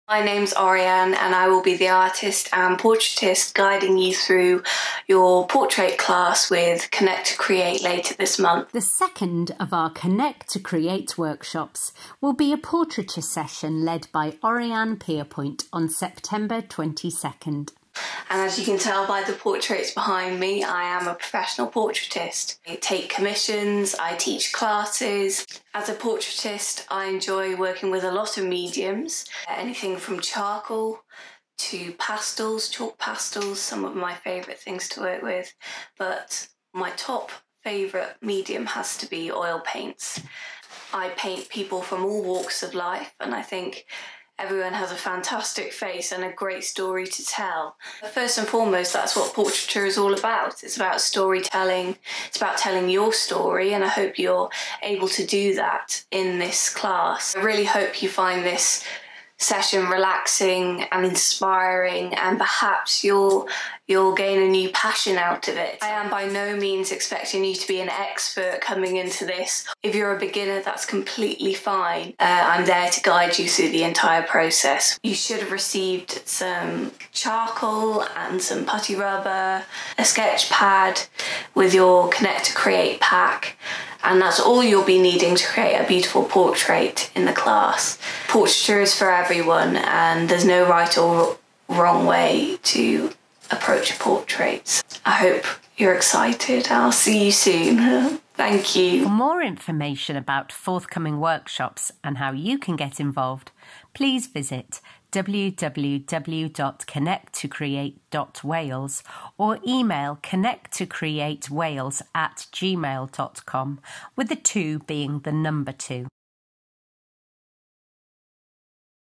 Connect2Create Wales Introduces Oriane Pierrepoint
Meet artist 🧑‍🎨  Oriane Pierrepoint to discuss the next FREE Connect 2 Create workshop which is Portraiture

The next workshop is coming on 22nd September at 2:00 pm. - Portraiture - Save The Date